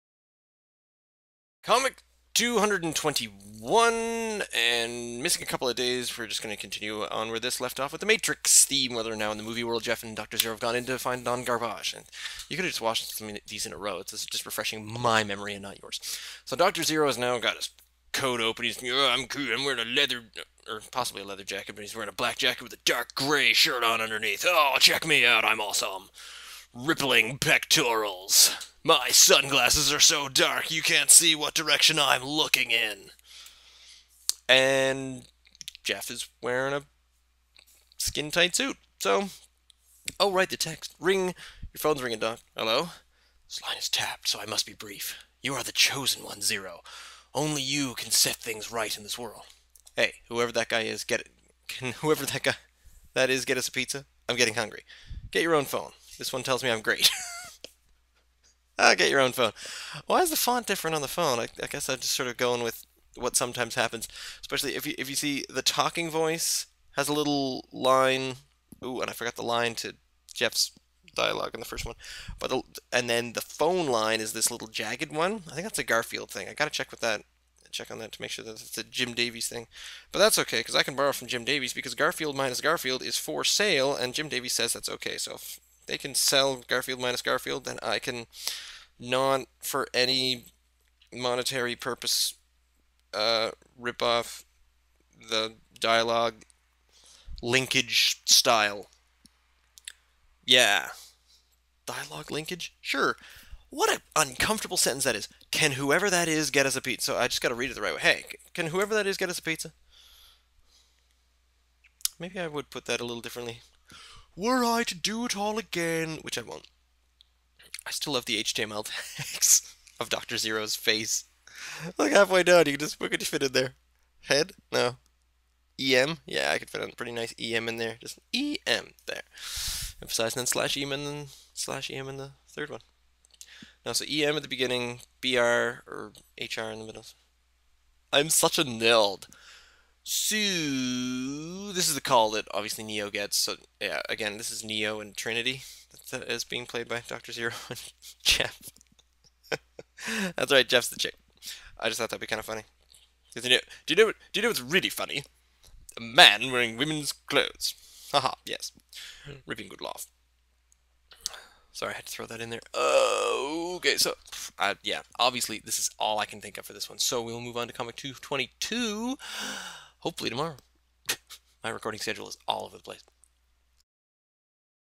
Intermission Commentary #221
Creator's commentary #221. In which I discuss ripping off Jim Davies some more.